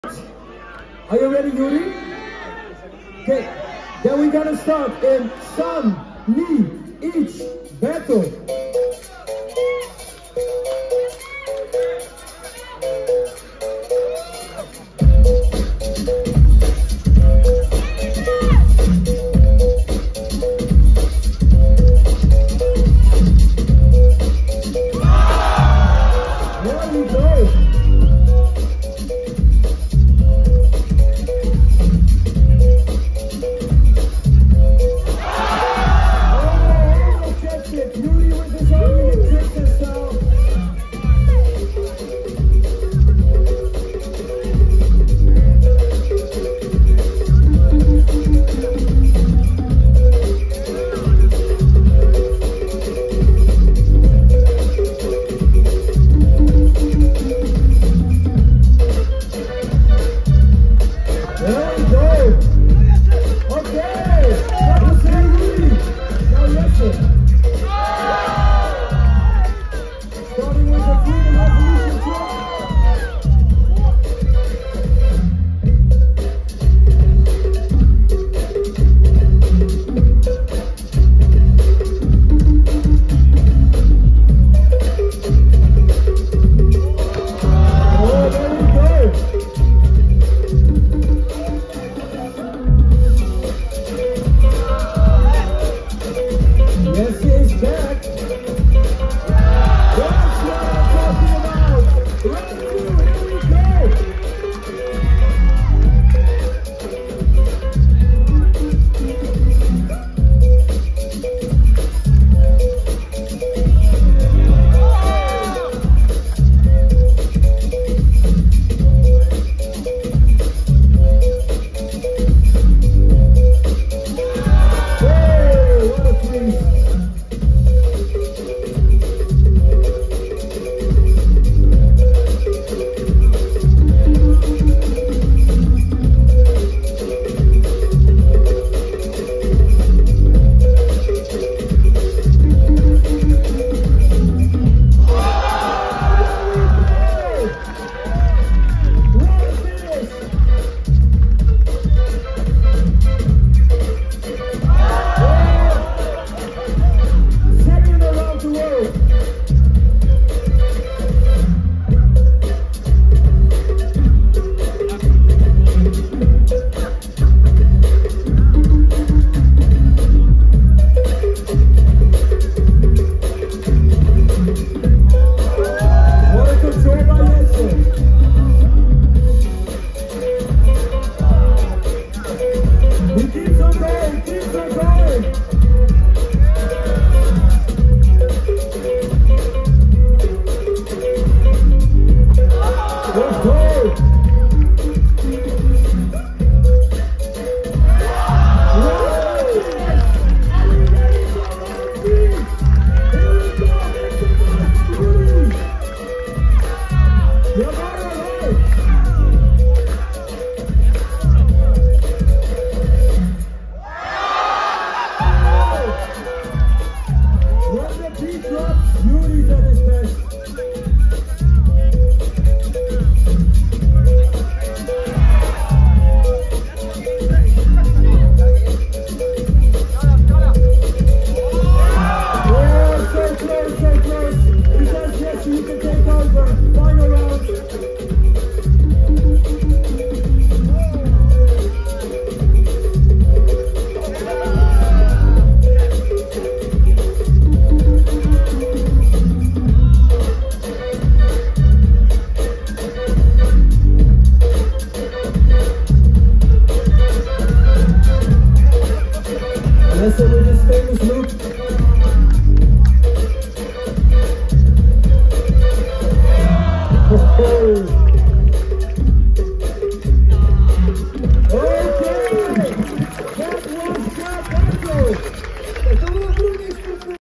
Yuri Kamalio vs Jesse | Quarterfinal | Super Ball World Freestyle Football Championship 2024
Yuri Kamalio (JPN) vs Jesse (NED) | Quarterfinal | Super Ball World Freestyle Football Championships 2024

Winner: Jesse (5-0)

Freestylers: @yuri_kamalio, @jessemarlet
Event: @superballlive
Filming: @yougotthecombo
Thumbnail: @azfotky